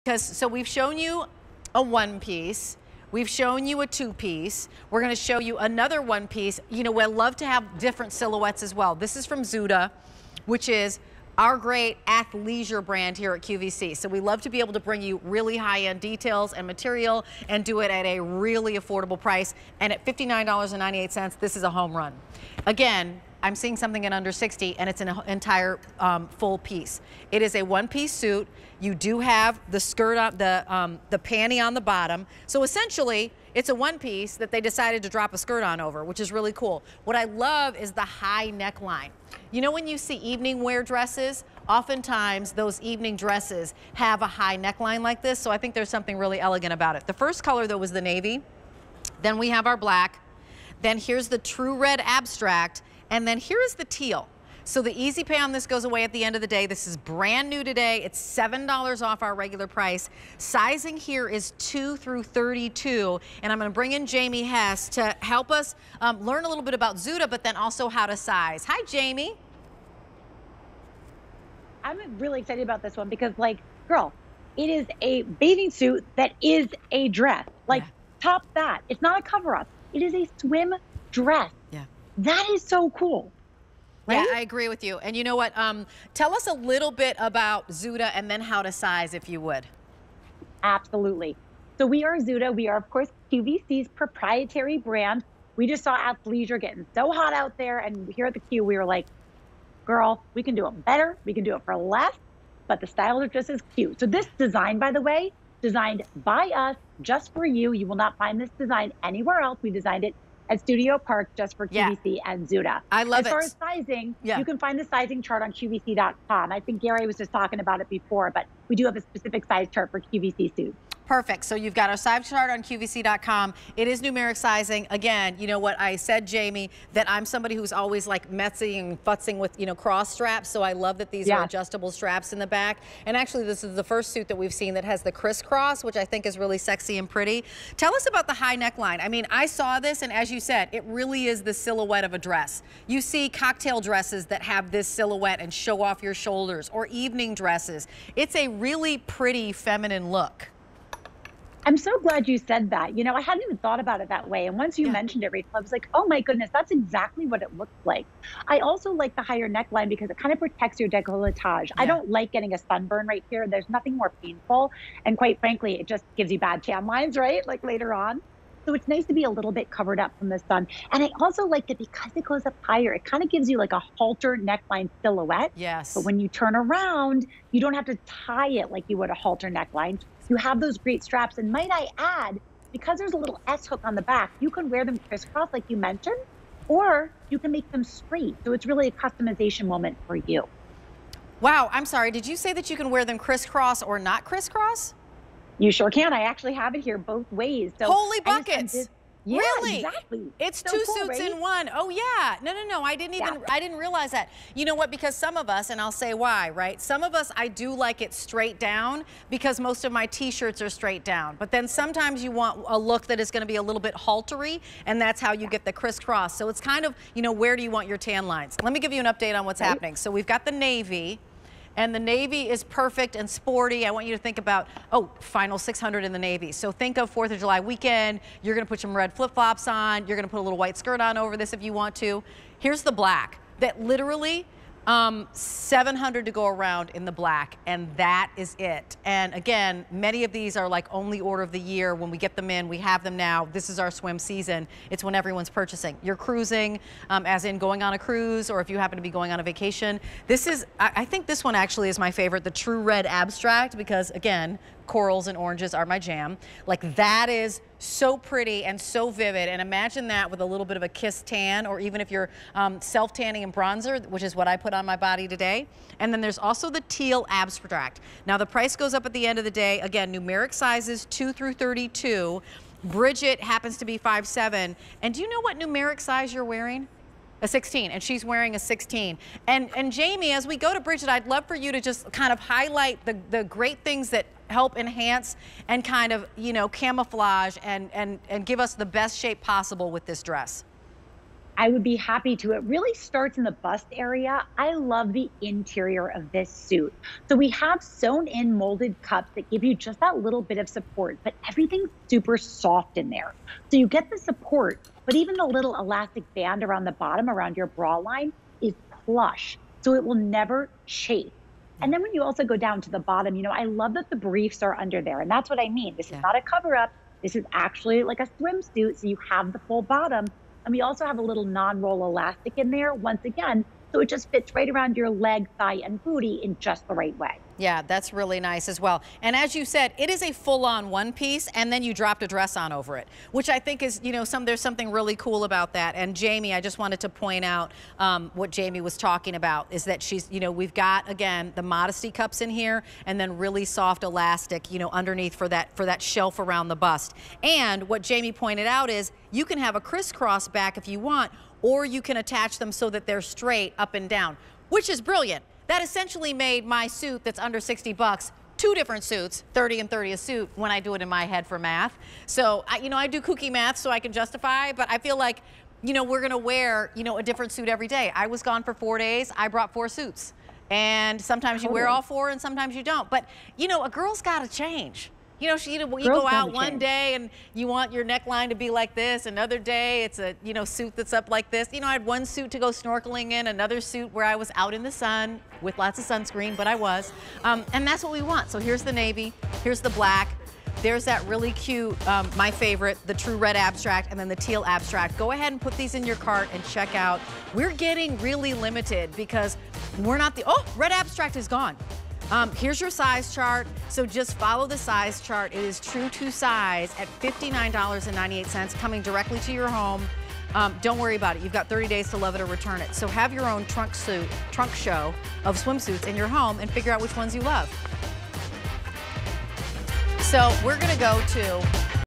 zuda Dress Hem One-Piece Swimsuit on QVC
zuda Dress Hem One-Piece Swimsuit

The plan: to stay active in and out of the water all season long. The look: a chlorine-resistant, color-fast one-piece equally as well-suited for laps as it is lounging. Thanks to movement-friendly slide slits, the sleek skirted style keeps you cruising from the shallow end to the swim deck, and the surf to the shore, without a hitch. (Now, who's up for some paddleball?!). From zuda&reg; Fashions.